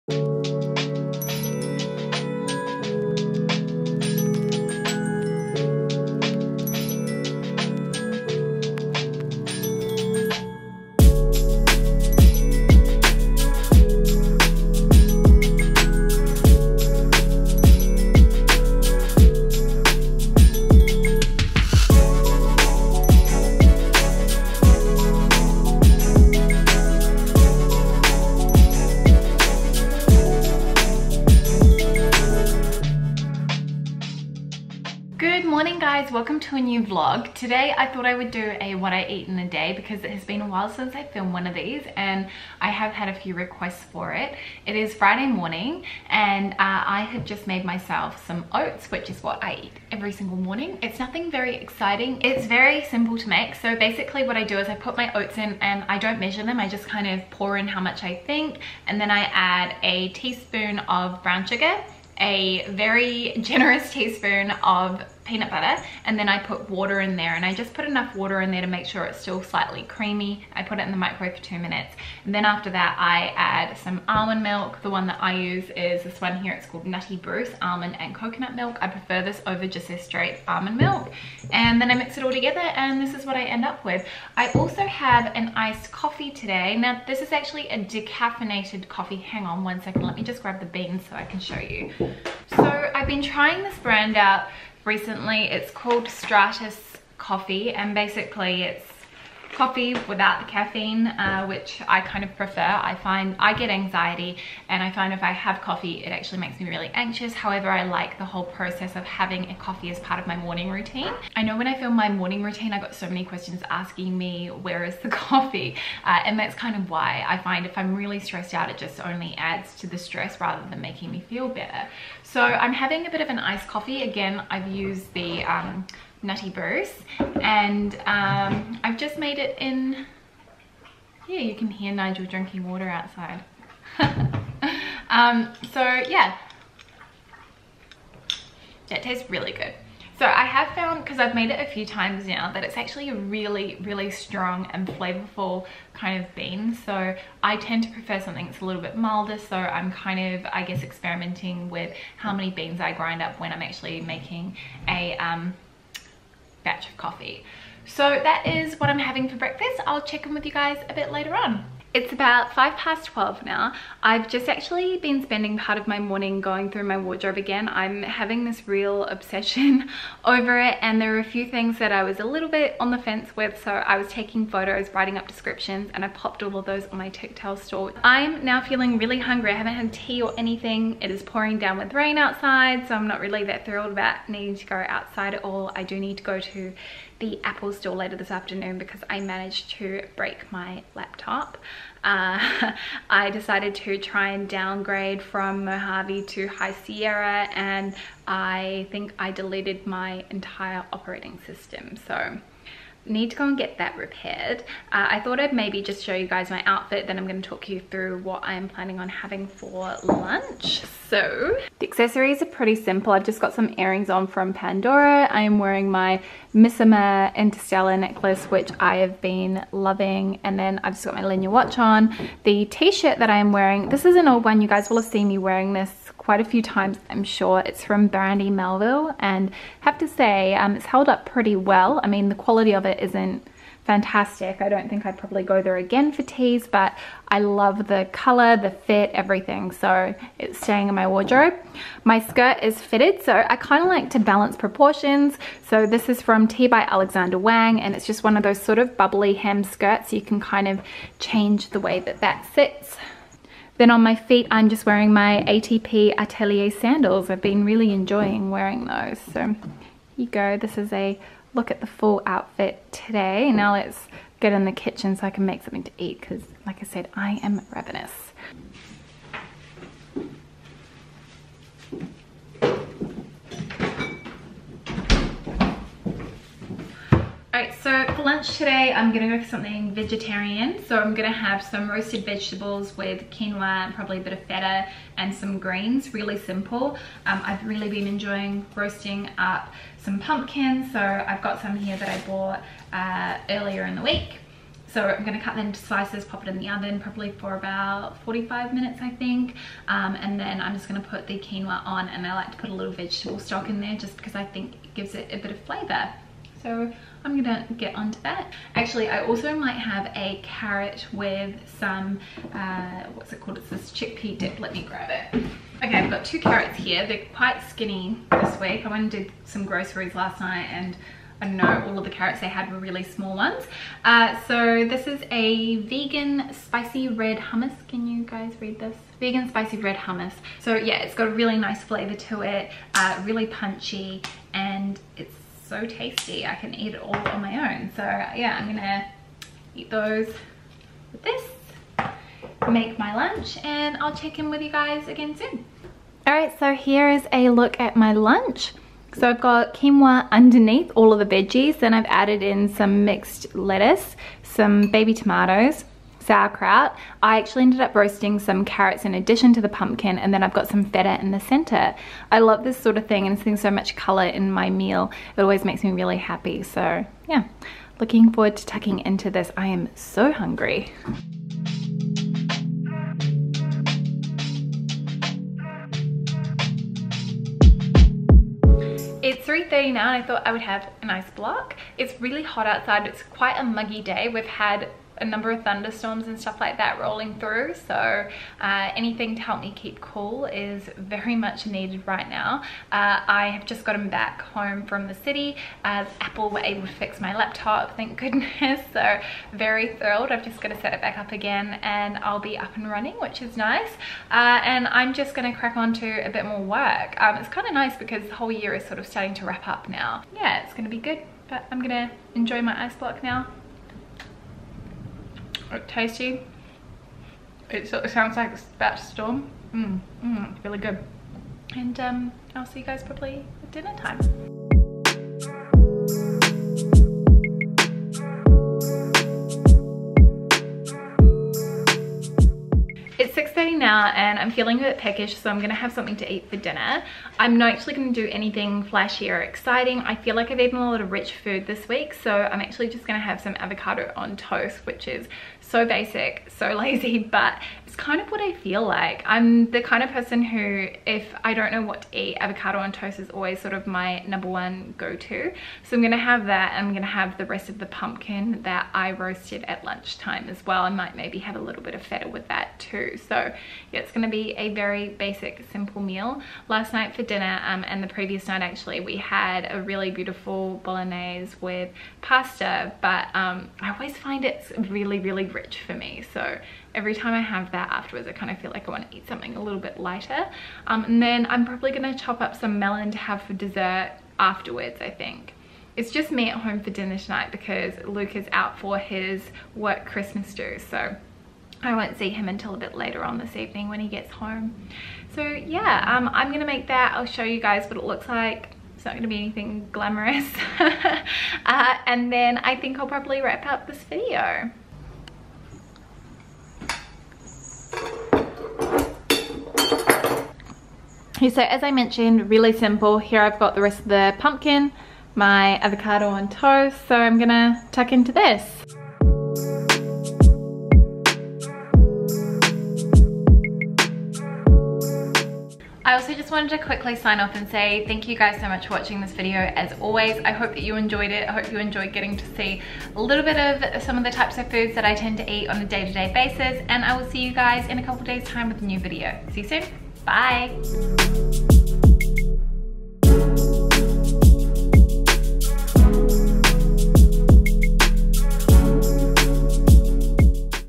What I Eat in a Day - Healthy Vegetarian/Plant Based | Mademoiselle
Today I’m sharing what I eat in a day. Obviously every day is different, and this turned out to be a (lazy) day of eating vegetarian/plant based foods. Hope you enjoy and anything mentioned will be linked below! x